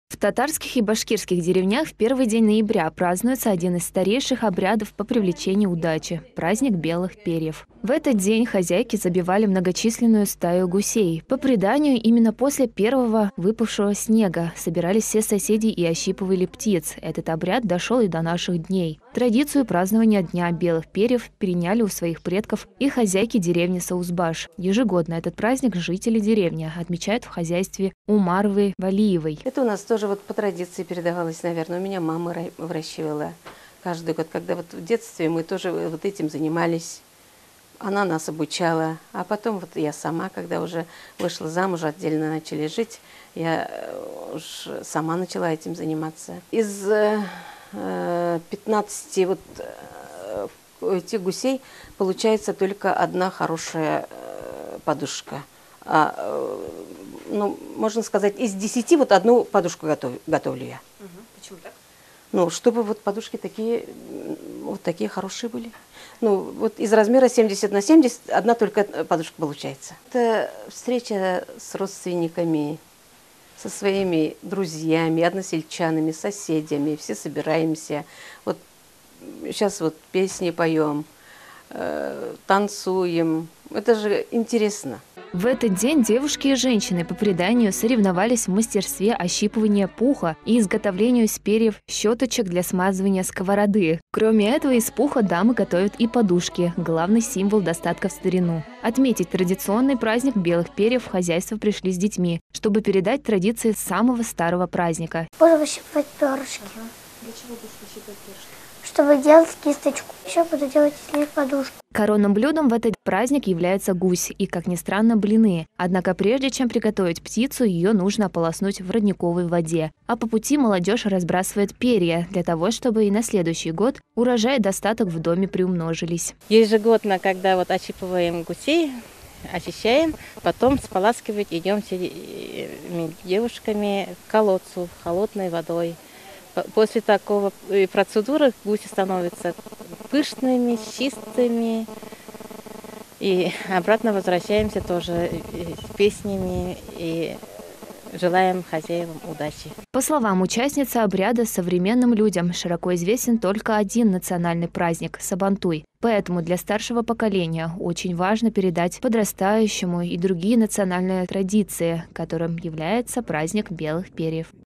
В Саузбаше отметили праздник белых перьев
Секретом древнего обряда по привлечению удачи поделились краснокамцы с нашим телеканалом. В деревне Саузбаш местные жители провели древний обряд «каз омэсе» - праздник белых перьев. О том, как привлечь удачу, расскажем далее в сюжете.